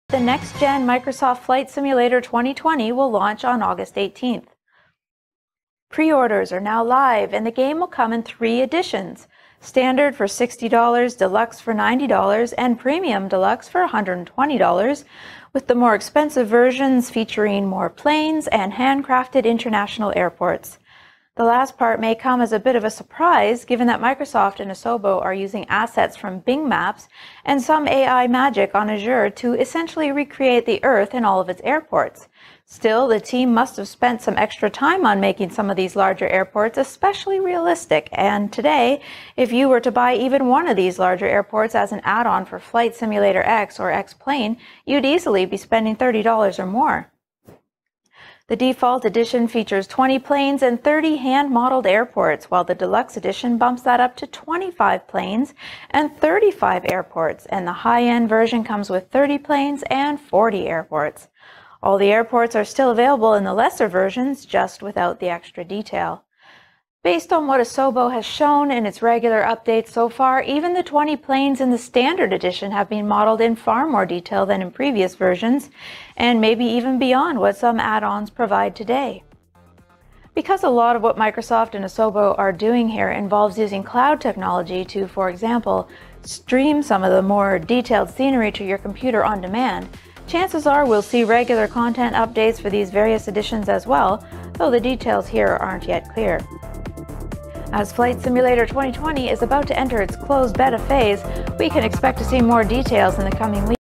Microsoft Flight Simulator 2020 will launch on August 18
After a series of closed alpha tests, Microsoft’s Xbox Game Studios and Asobo Studio have announced that the next-gen Microsoft Flight Simulator 2020 will launch on August 18. Pre-orders are now live, and the game will come in three editions: standard for $60, deluxe for $90 and premium deluxe for $120, with the more expensive versions featuring more planes and handcrafted international airports.